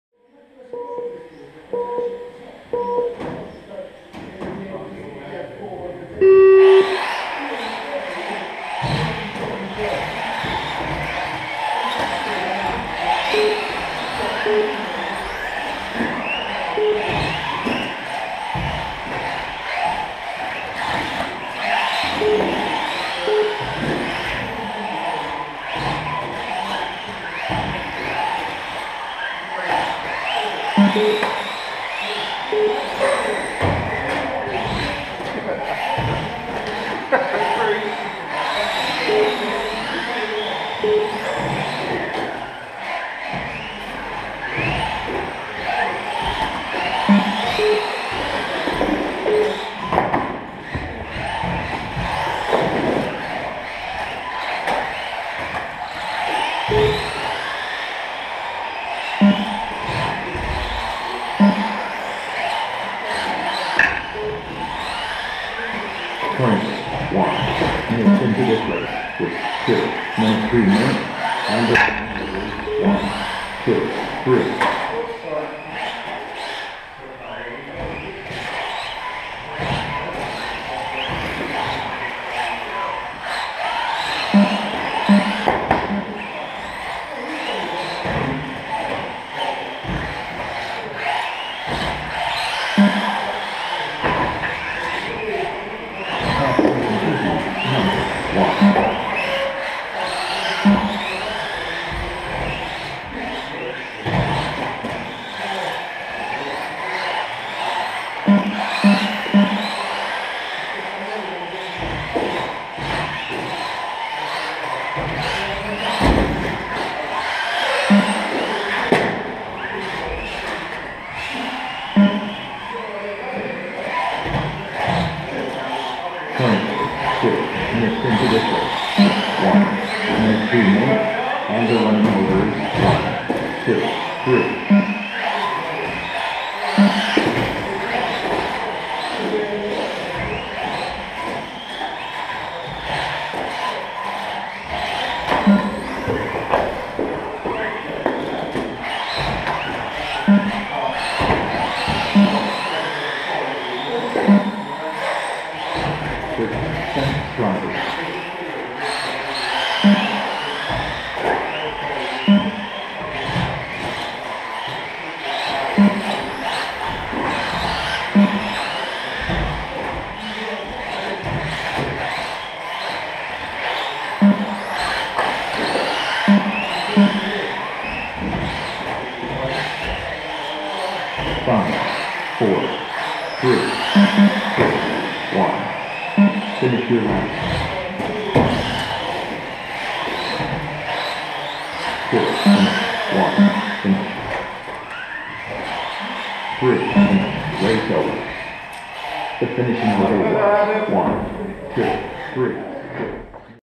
TRCMCC - C Final - 25/05/16
C Final @ TRCMCC in Tipton St John near Exeter Devon on 25/05/16